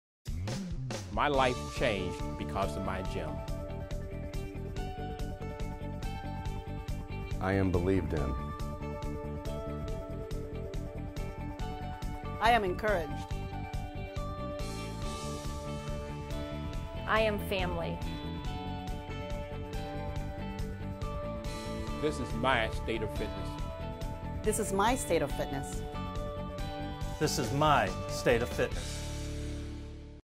State of Fitness Sense of Community Commercial
At State of Fitness our members are believed in! They are encouraged! They are family! This is their State of Fitness! What is your State of Fitness?